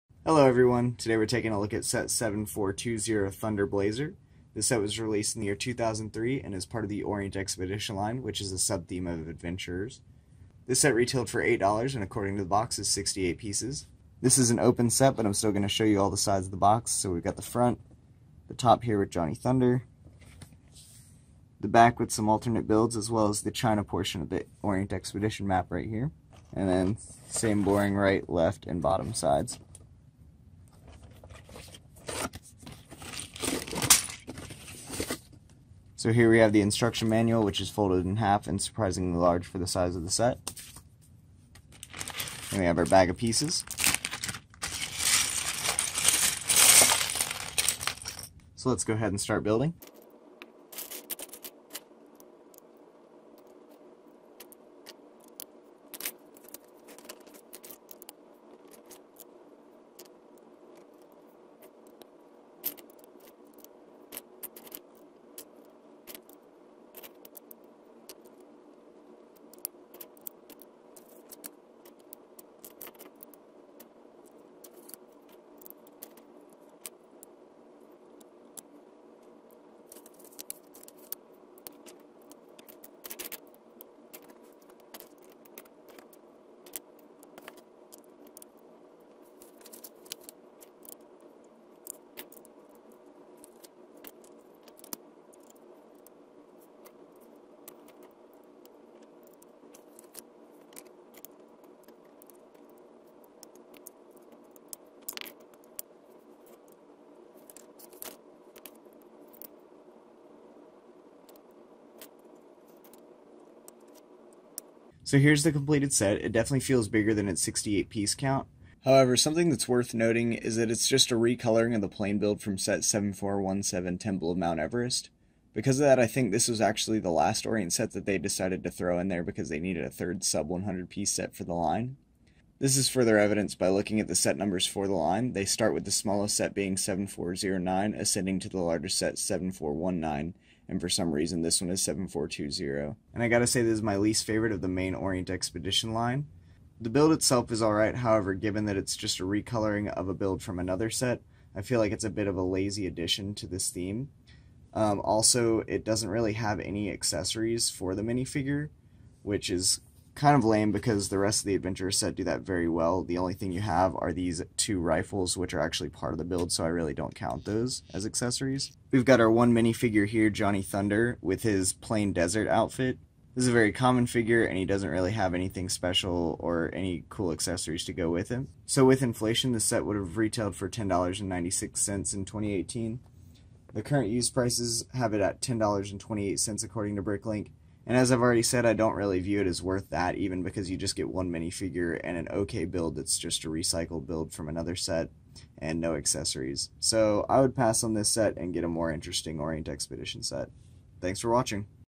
LEGO Orient Expedition 7420 Thunder Blazer - Review 2003
I review this set from the Orient Expedition line